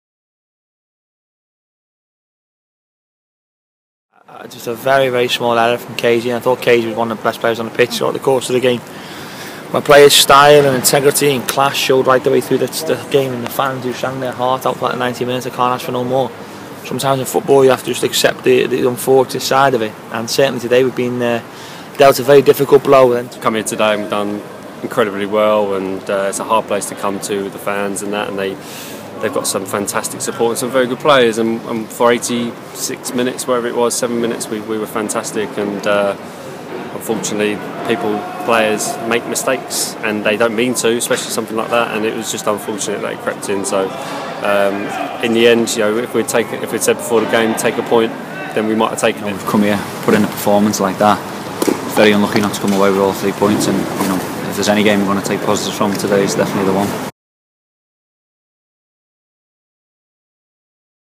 INTERVIEW: Robinson, Bowditch and Potter after Leeds
Karl Robinson, Dean Bowditch and Darren Potter react to the Dons' 1-1 draw with Leeds United at Elland Road...